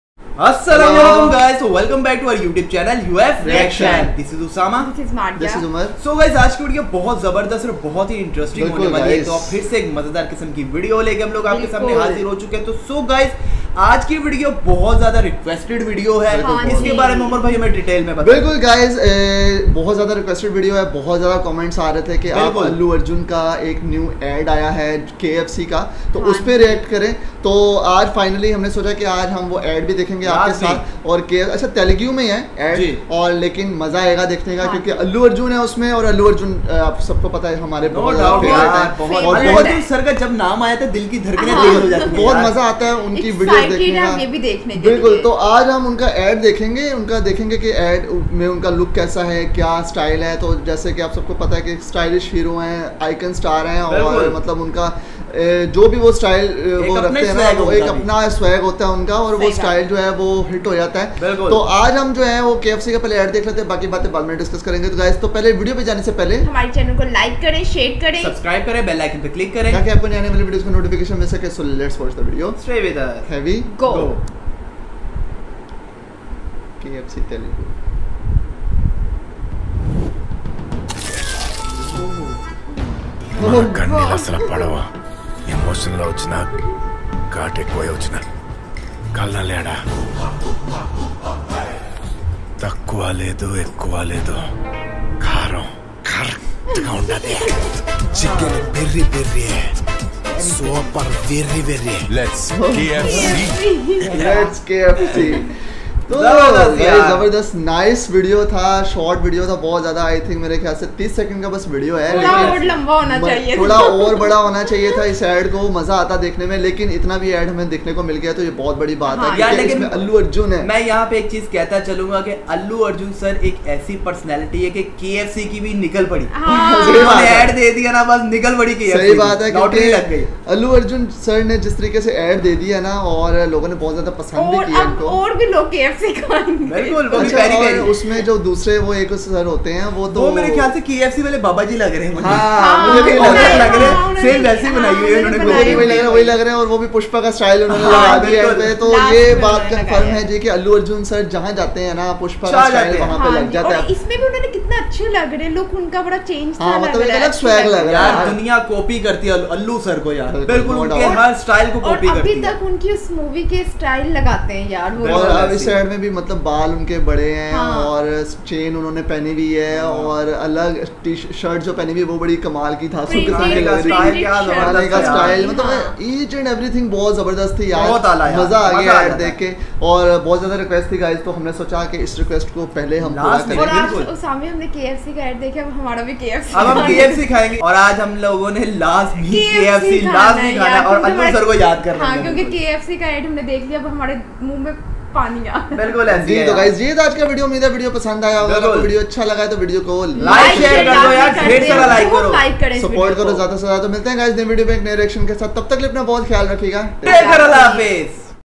Pakistani Reacts To Icon Star Allu Arjun Latest KFC ad 2022| KFC Advertisement
UF_Reaction

credits
channel name:NS Entertainment